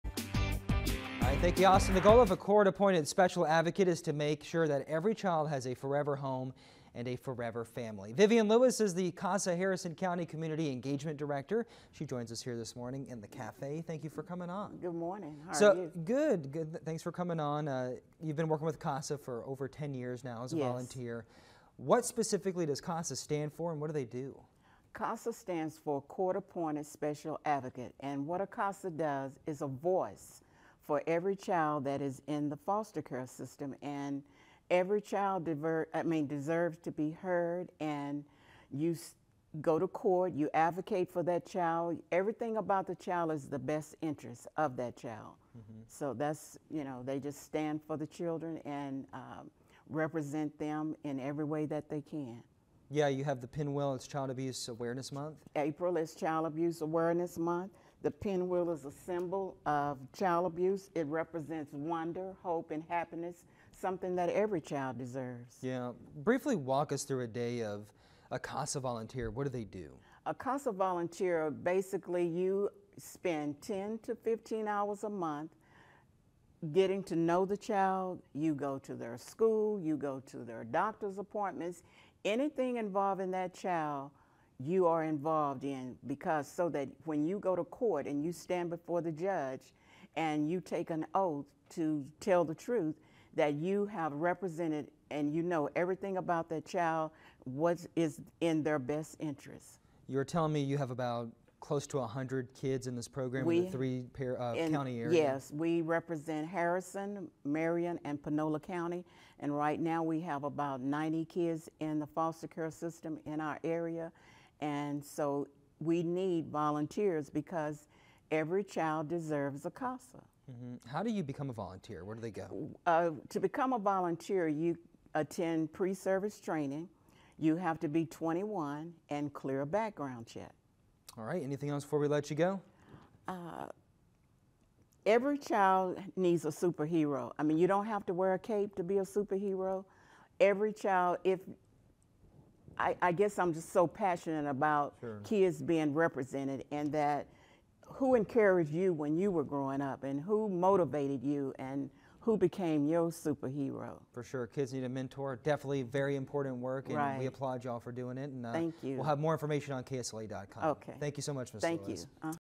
How to become a CASA volunteer
The goal of a court appointed special advocate is to make sure that every child has a forever home and forever family.